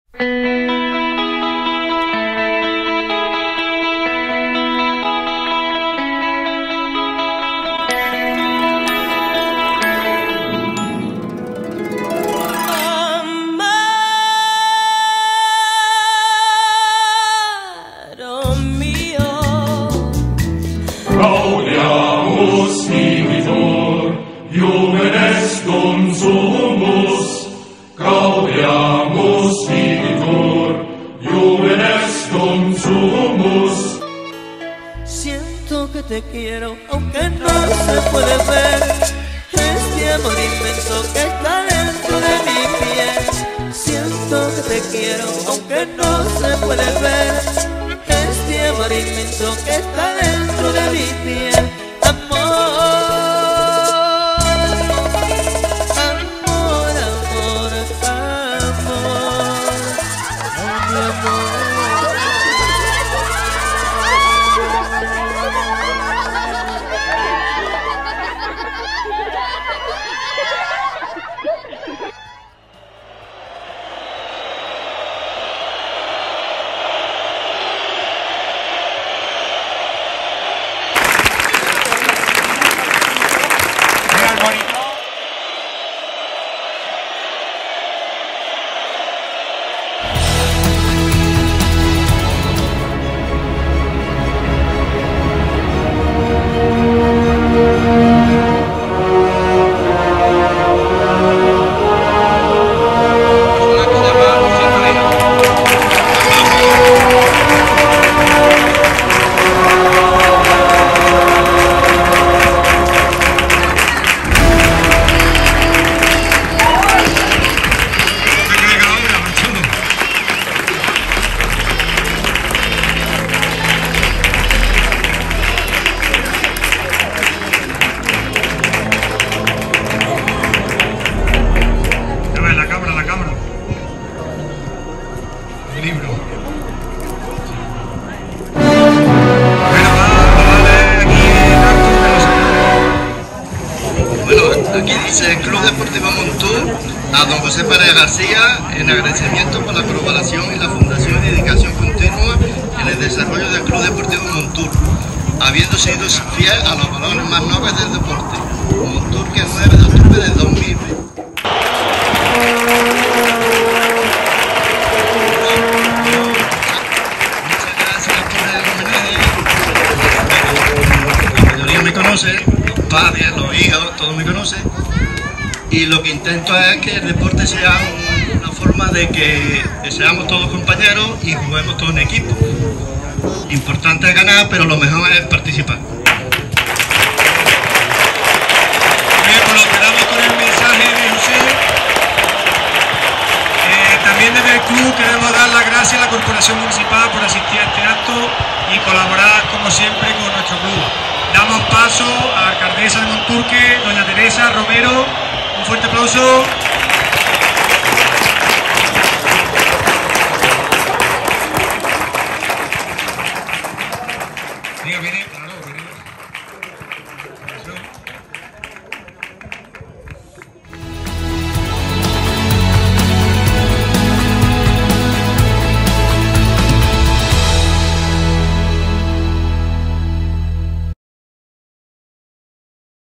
Homenaje a José Pareja del CF Monturk
El CF Monturk rindió Homenaje a Don José Pareja García el 9/10/2022 en agradecimiento por su colaboración en la fundación del Club Deportivo Monturk y por su dedicación continua al desarrollo del mismo.